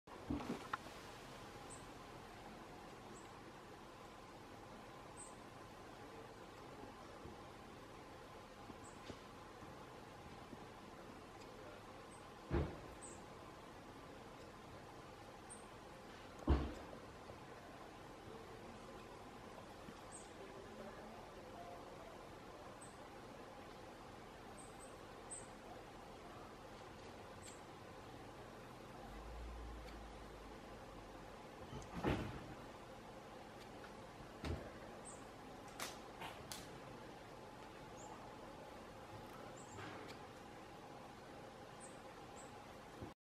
Brown Creeper (Certhia americana) Getting a Drink - Armstrong Redwoods State Park
Brown Creeper (Certhia americana) Getting a Drink - Armstrong Redwoods State Park, Sonoma County, California